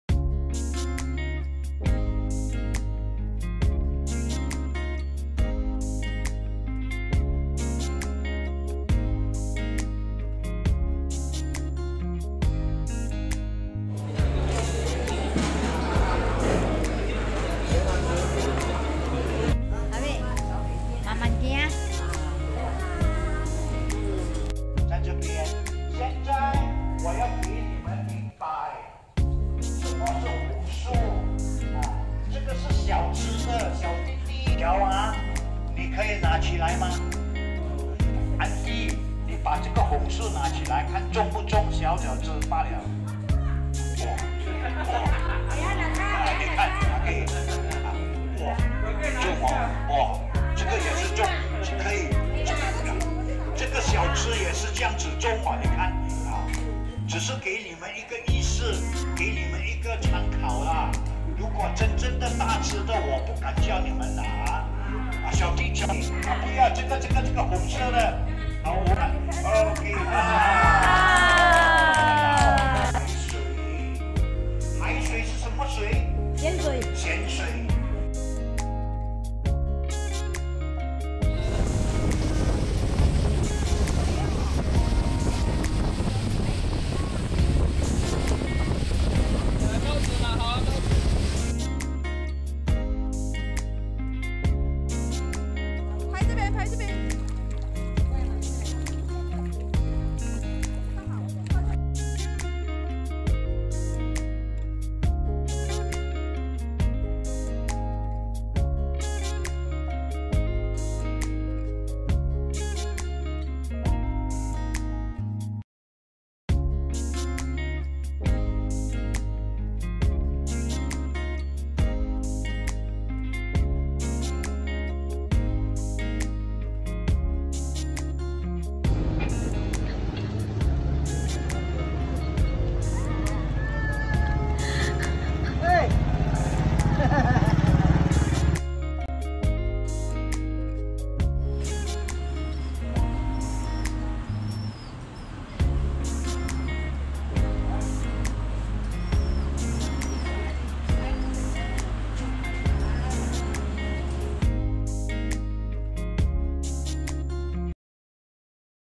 Kuala sepetang十八丁:生态游
阳光正好☀️ʜᴀᴠᴇ ᴀ ɴɪᴄᴇ ᴅᴀʏ︎
记录疫情过后的第一次出游  ​